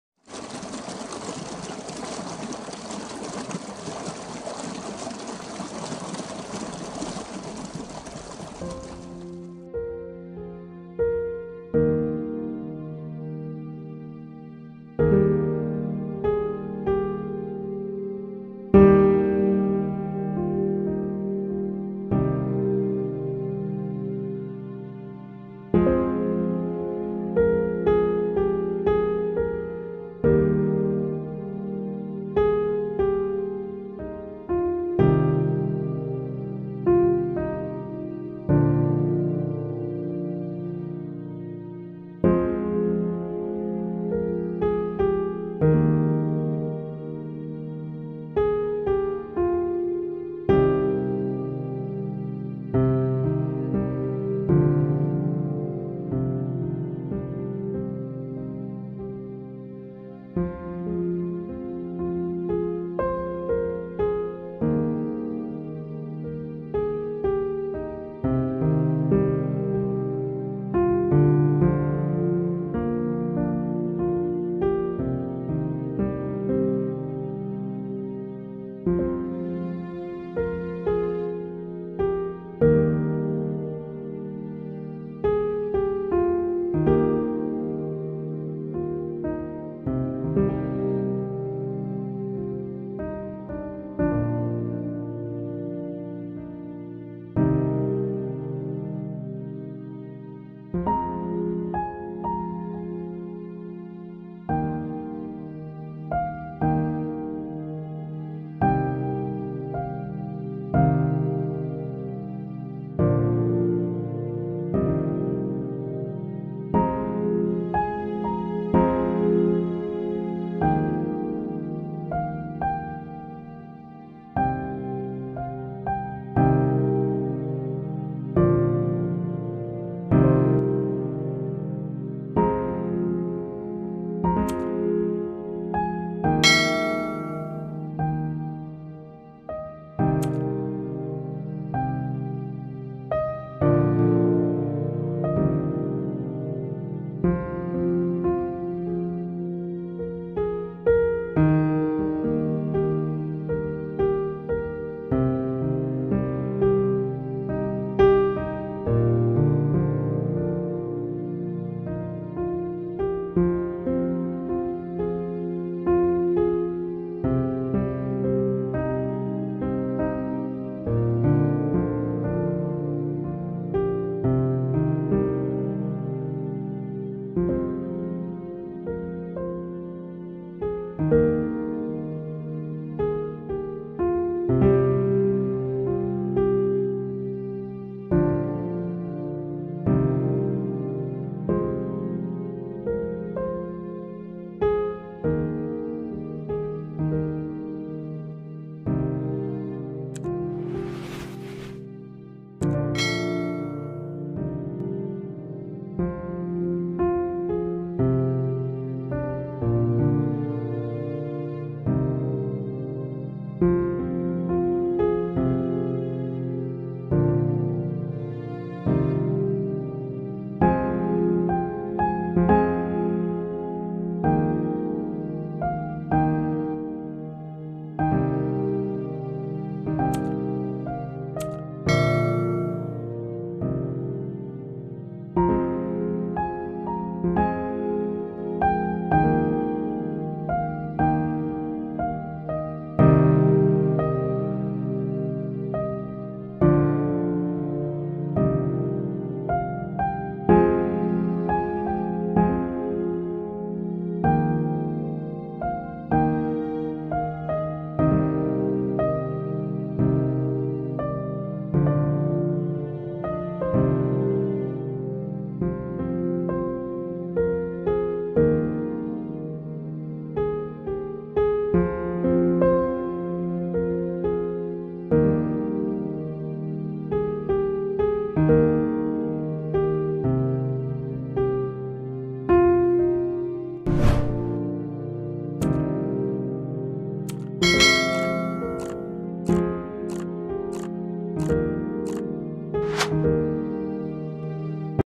Relax time walking through Jaffna vegetables farm with Melody
This video has been taken in Kokuvil GN in Jaffna District and music composed by T.Thushanthan.We hope that,this video make happy and relax mind.